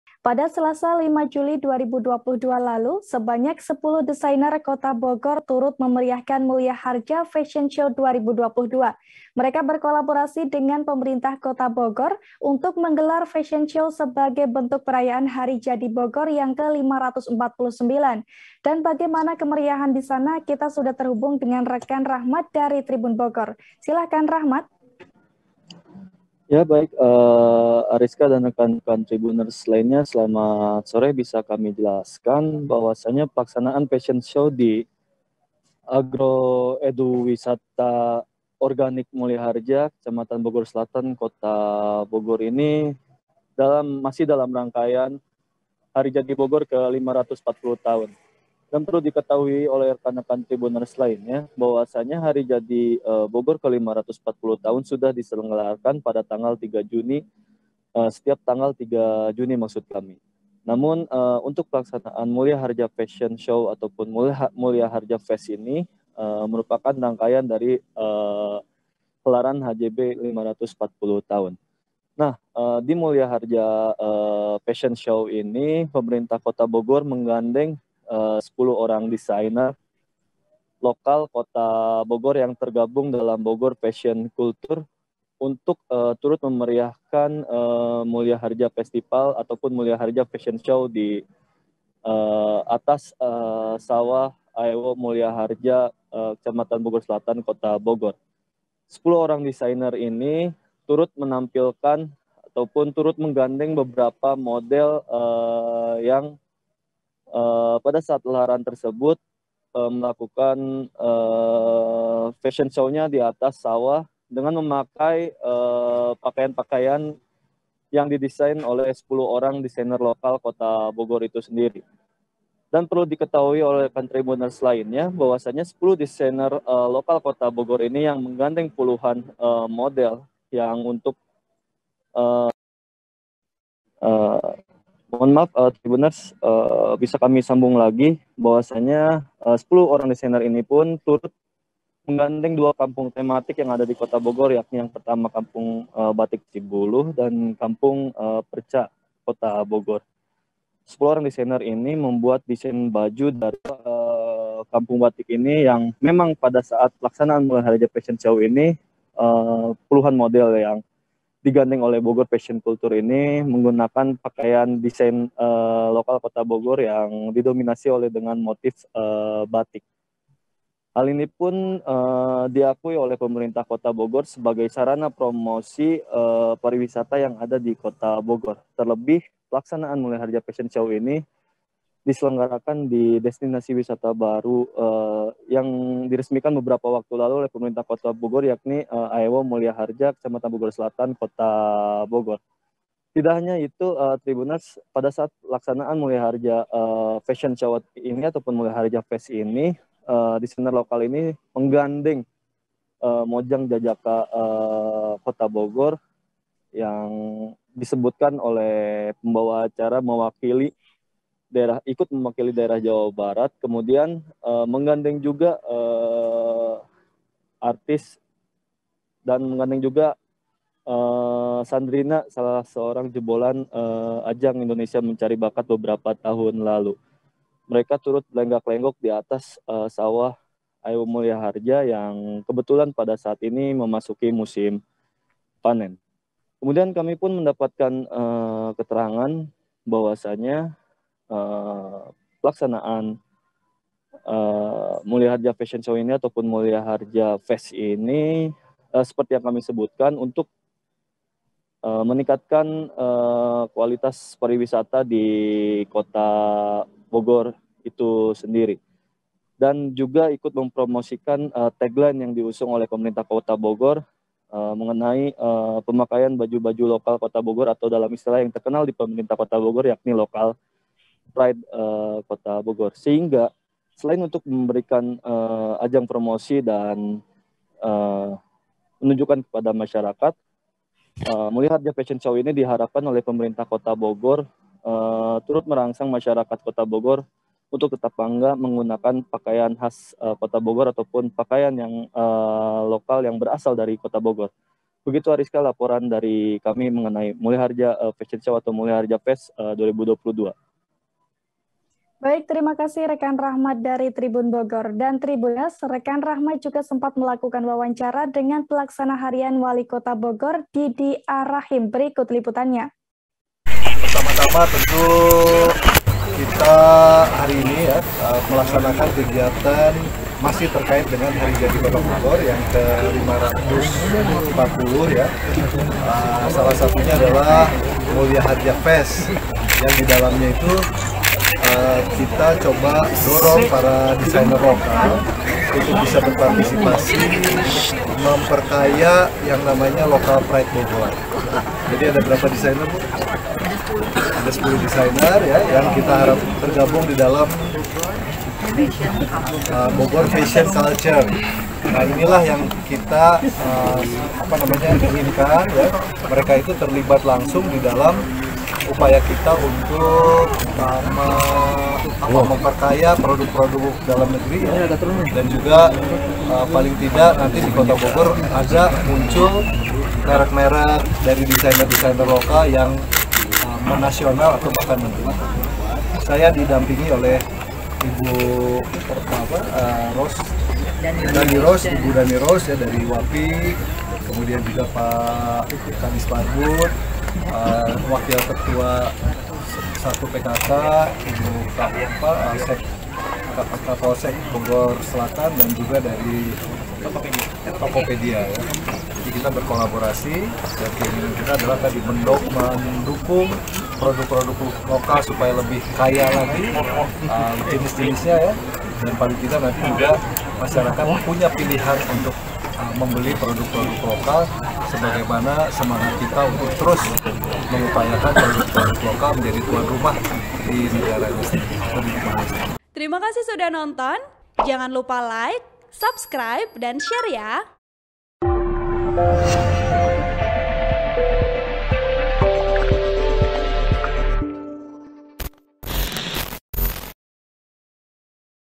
Intip Kemeriahan Mulyaharja Fashion Show 2022, Kenalkan Produk Lokal Kota Bogor dari Atas Sawah
TRIBUN-VIDEO.COM - Tribunners, pada Selasa (5/7/2022) Mulyaharja Fashion Show 2022 digelar di kawasan Agro Edu Wisata Organik (AEWO) Mulyaharja.

Dalam acara ini, ada sebanyak 10 desainer Kota Bogor yang turut memeriahkan.

Mereka yang tergabung dalam Bogor Fashion Culture ini berkolaborasi dengan Pemerintah Kota Bogor untuk menggelar fashion show.

Diketahui, acara ini dilakukan sebagai bentuk perayaan Hari Jadi Bogor yang ke-540.

Berbagai desain baju dengan bermotifkan batik, turut dipamerkan oleh 10 orang desainer Kota Bogor ini.

Mulai dari Sandrina IMB, hingga mojang jajaka Kota Bogor turut digandeng untuk memperkenalkan produk-produk lokal Kota Bogor.

Mereka berlenggak-lenggok di atas sawah yang mulai menguning di kawasan AEWO Mulyaharja.

Sementara itu, pelaksana harian (Plh) Wali Kota Bogor Dedie A Rachim mengatakan, kegiatan ini bertujuan untuk mengenalkan produk lokal Kota Bogor. (*)